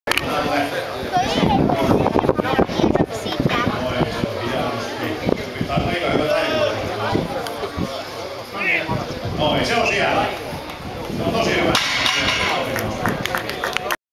Apollon axle c&p 120kg
Topi Frifeldt, Eura osmantupa strongman open 2010.